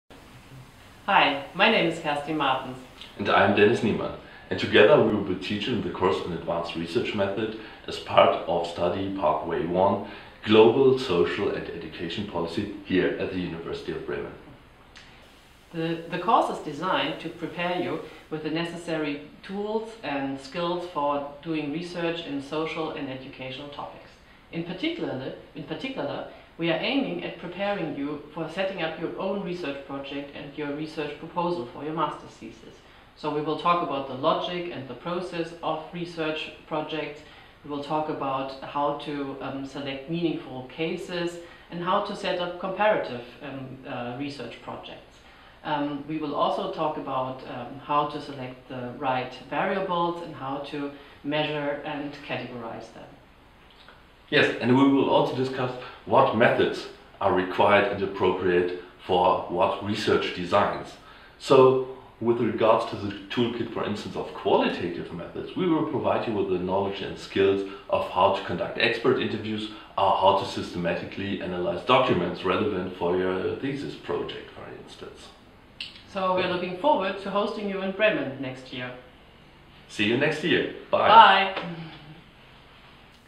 Advanced research methods (UB)
The Professors Kerstin Martens and Dennis Niemann give us some clues about the Advanced research methods course being taught at the University of Bremen.